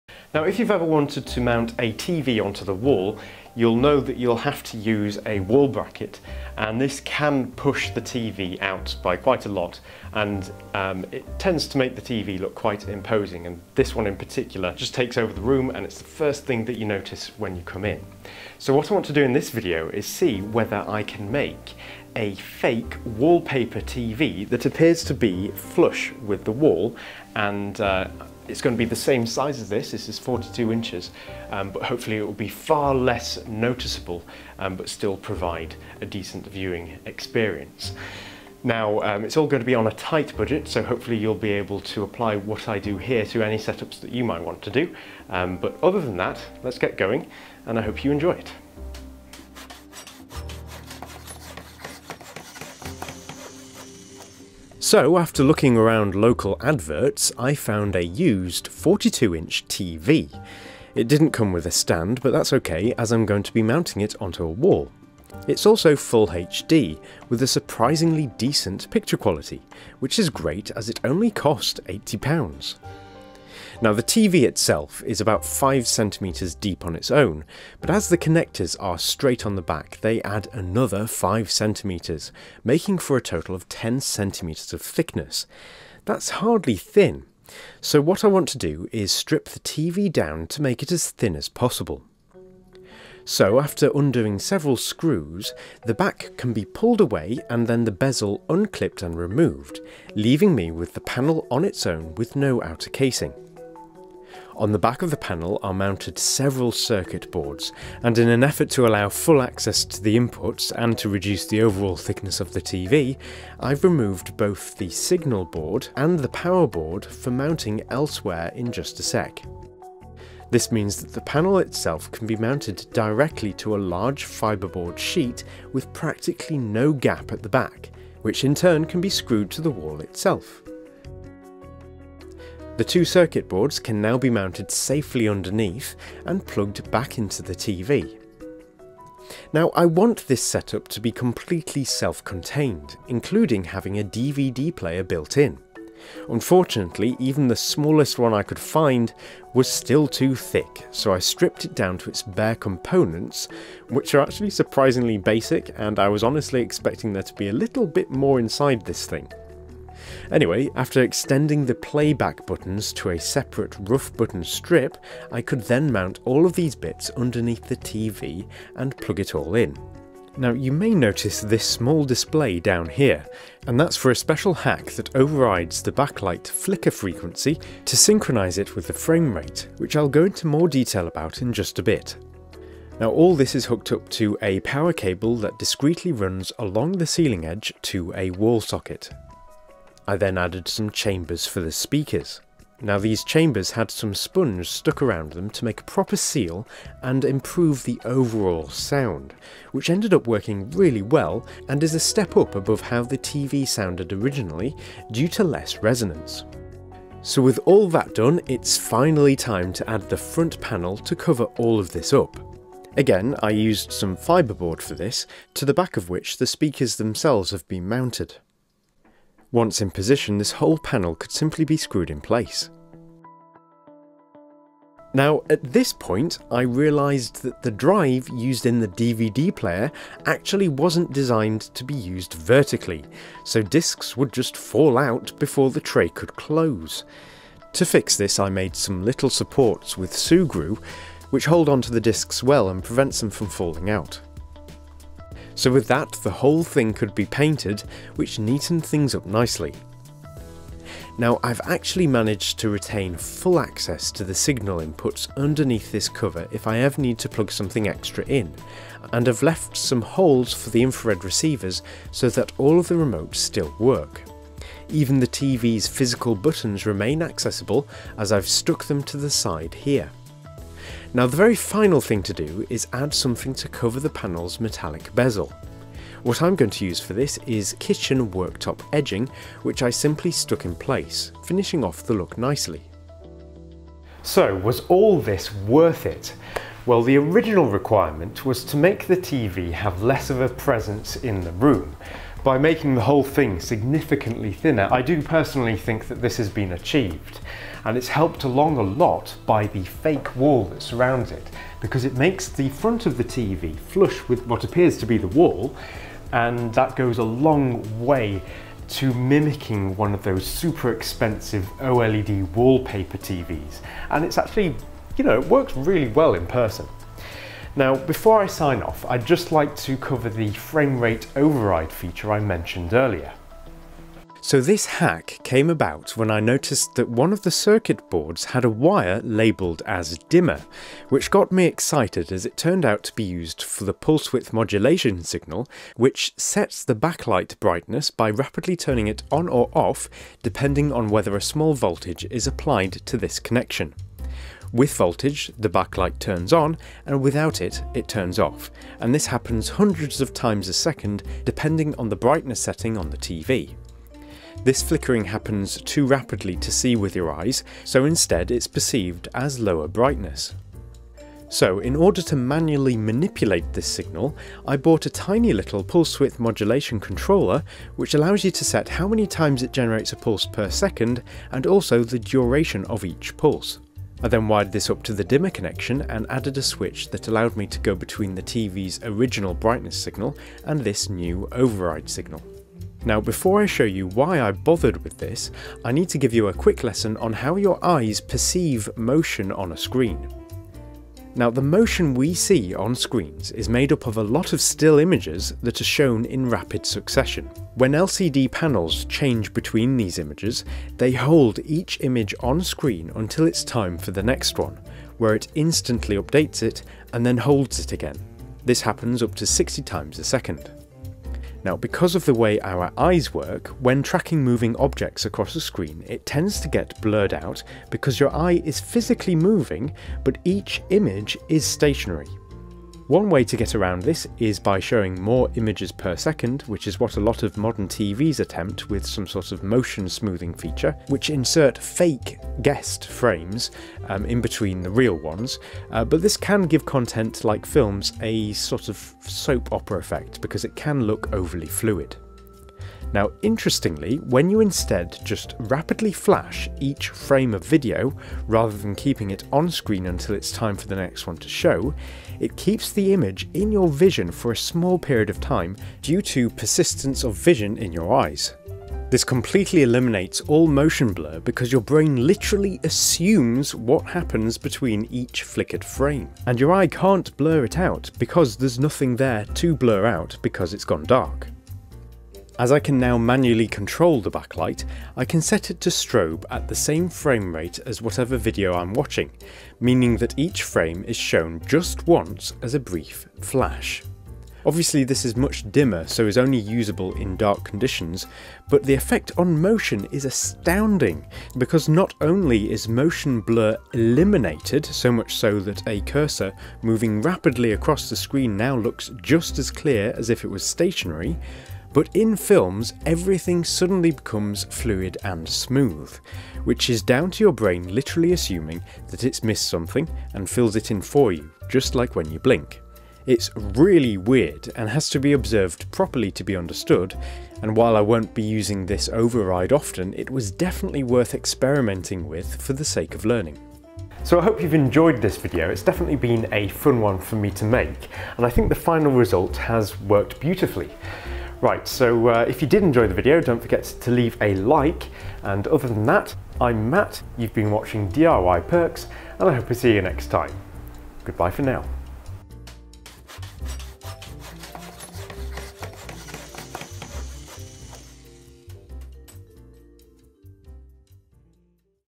Building a homemade 'wallpaper TV'
In this video I attempt to build a home-made 'wallpaper TV' on a shoestring budget, and the result is surprisingly good!

UFO motion tests are from BlurBusters.com and the video shown on screen is from the amazing scenery video 'Patagonia 8k'